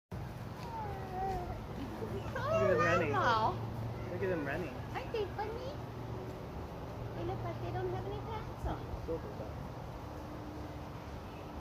He's not wearing pants.
Zola, B'wenzi and Shana were playing catandmouse with Juba today.

7 October 2023